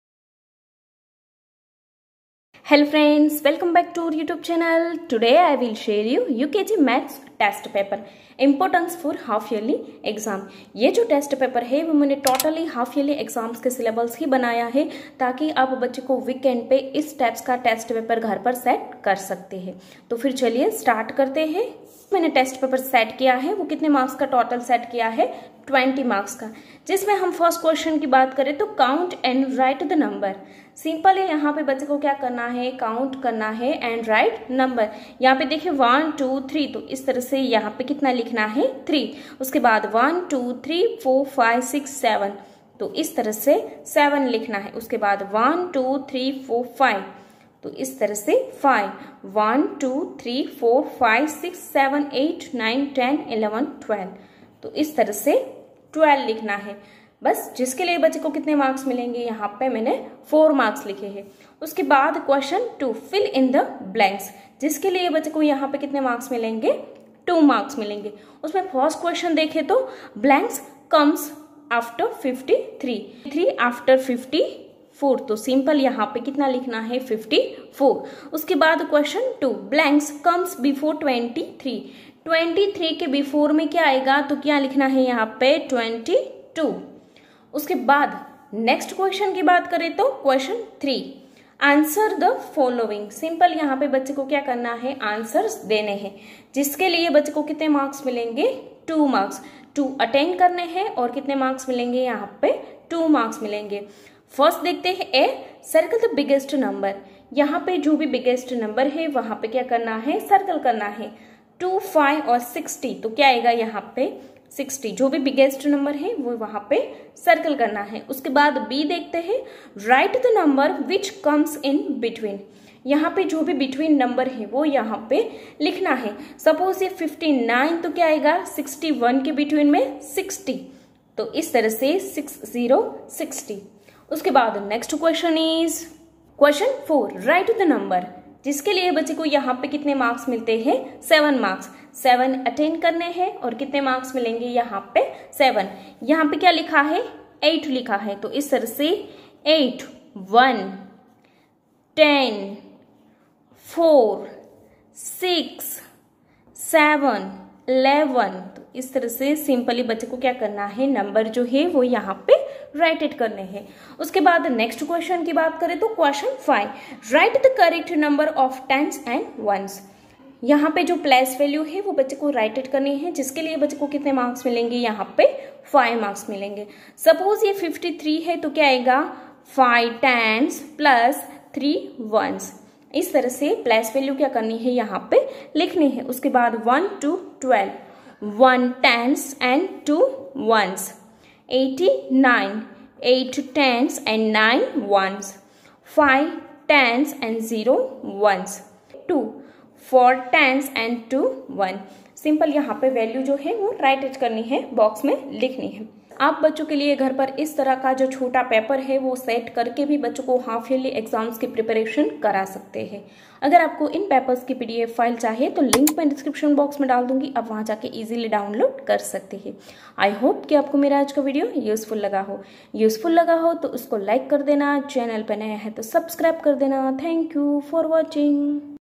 UKG Maths Test Paper 2023-24/UKG Maths Test Paper For Half yearly Exam preparation/UKG Class Maths
Hello friends,
well come back to our youtube channel
 vyomonlineclasses

About this video:
In this video though I'll share you ukg class kids maths test for half yearly exam preparation in this test I'll set some different Questions Include so plz watch video till to end...